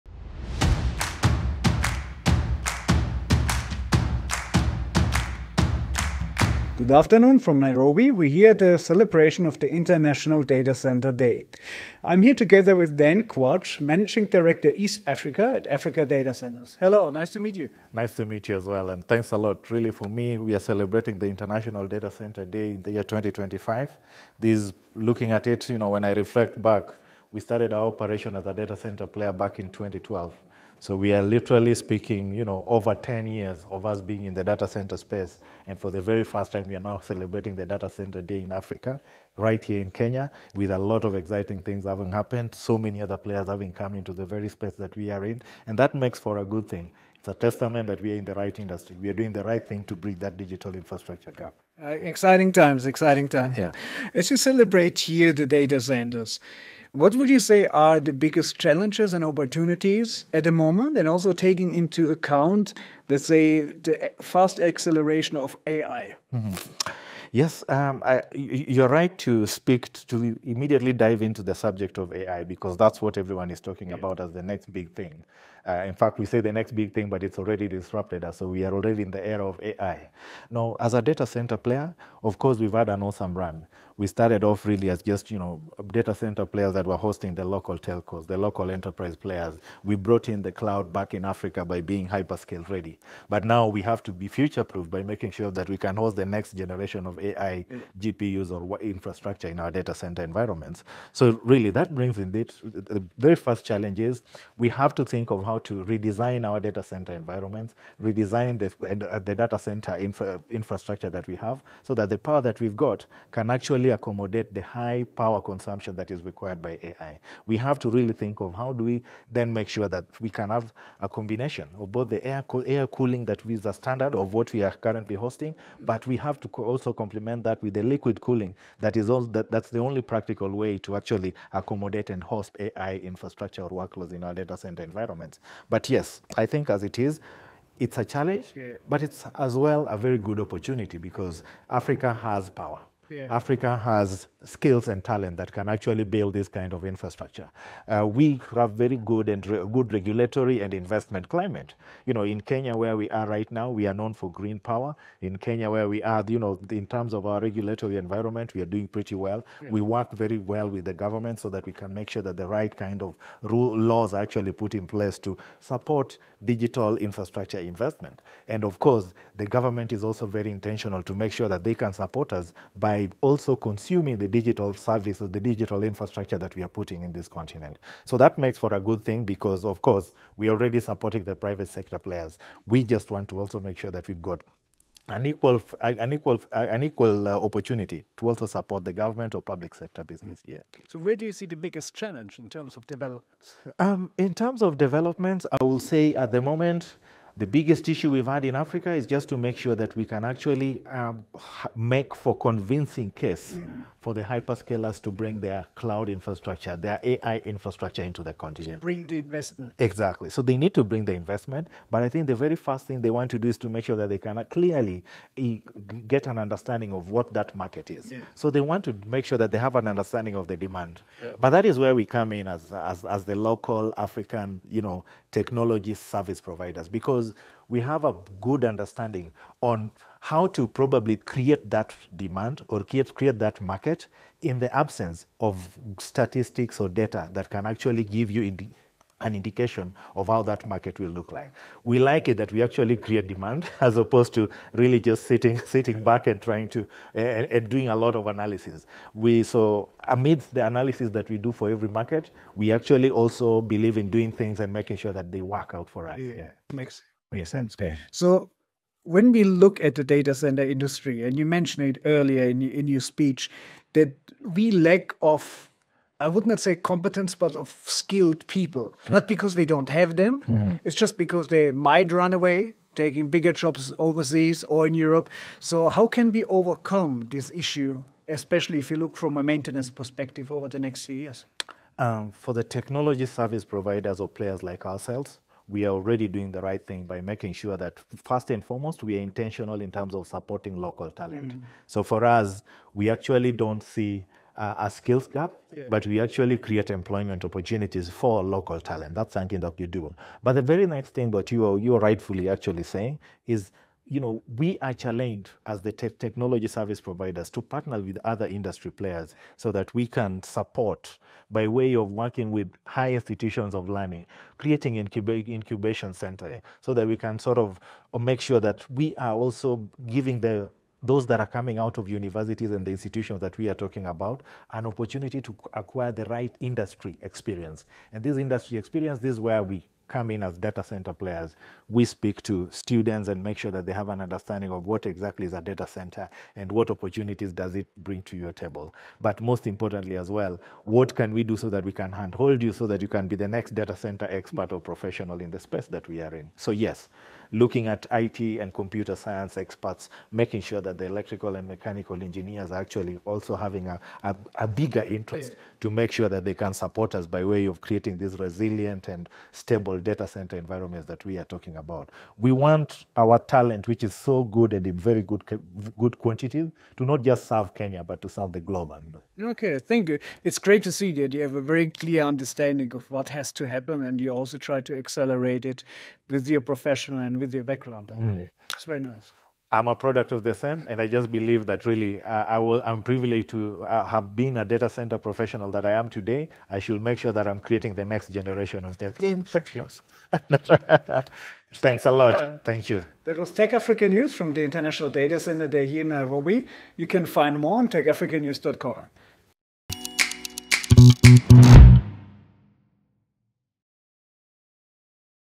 Africa Data Center’s Role in Powering AI, Innovation, and Digital Growth
The event brought together key industry players to discuss the state of Africa’s data center industry, its evolution over the past decade, and the opportunities presented by emerging technologies like AI and cloud computing.

We met with Dan Kwach, Managing Director - East Africa Region at Africa Data Centres, who shared valuable insights into the sector’s growth, the hurdles it faces, and what the future holds for Africa’s digital infrastructure.